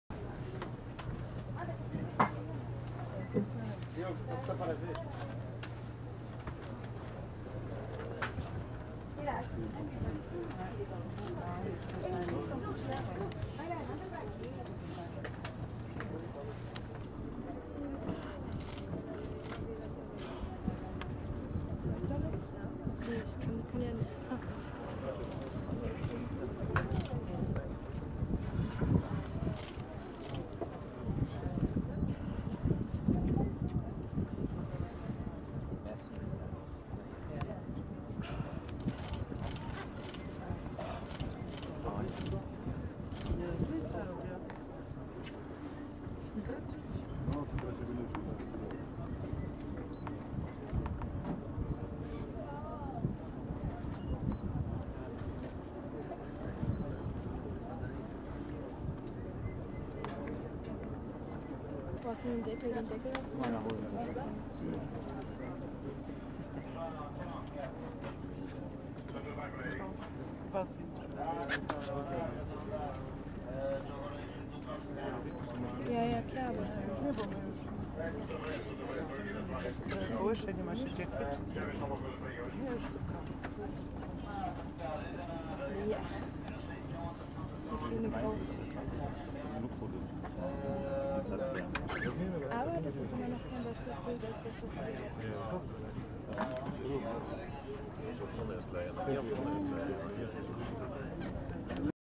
spotting pilot whales
We went on a catamaran trip and saw some pilot whales :)